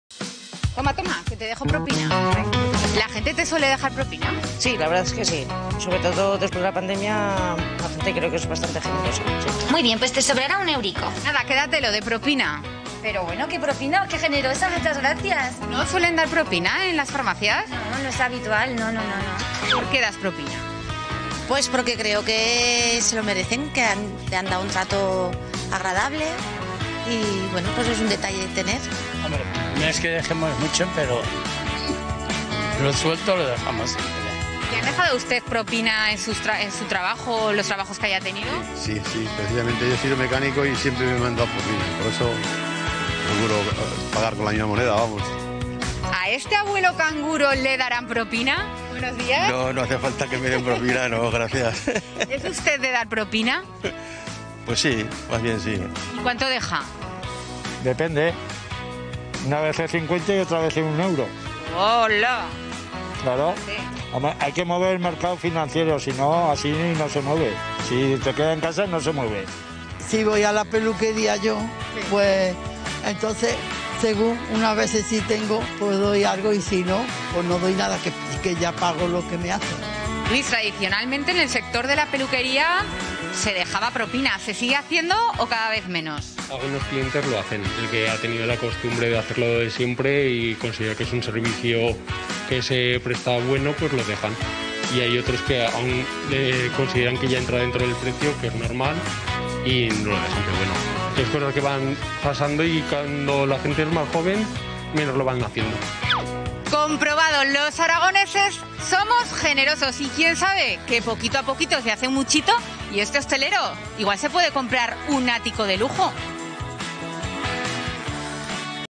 ¿Suelen los aragoneses dejar propina?
La propina era una costumbre que antes estaba muy extendida. Pero, ¿y ahora? ¿Siguen dejando propina los aragoneses?